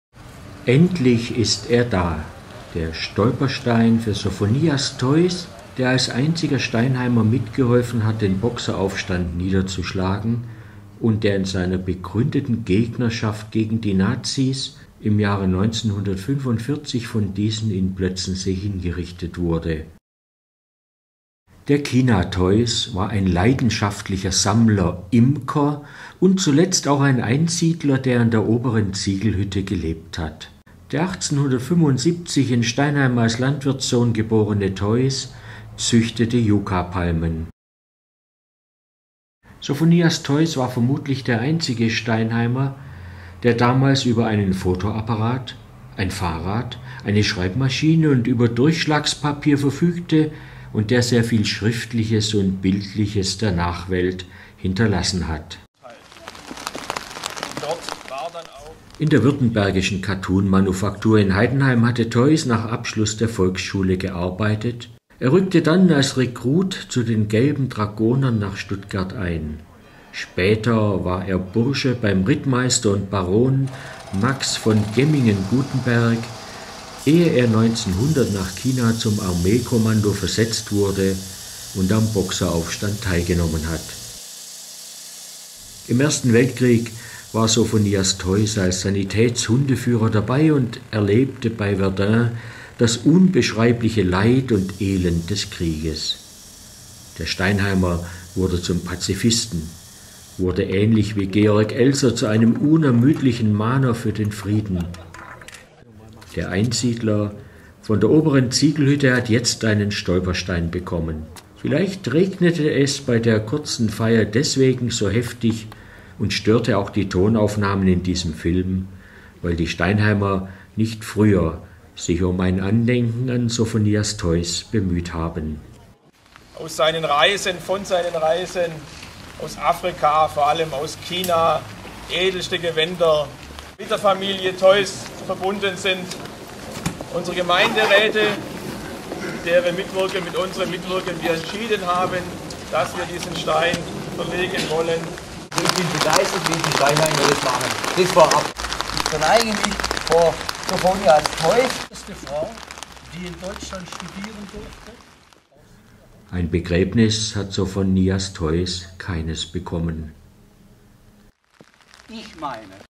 Stolperstein für Sofonias Theuß
Dem von den Nazis in Plötzensee 1945 hingerichteten Imker und Einsiedler Sofonias Theuß wurde an der Kirchstraße in Steinheim ein Stolperstein gesetzt. Er war vermutlich der erste Steinheimer, der über ein Fahrrad, eine Schreibmaschine und einen Fotoapparat besaß und bemerkenswerte Aufzeichnungen der Nachwelt hinterließ. Sofonias Theuß verdankt Steinheim eine umfangreiche Sammlung aus der Zeit des Boxer-Aufstandes in China.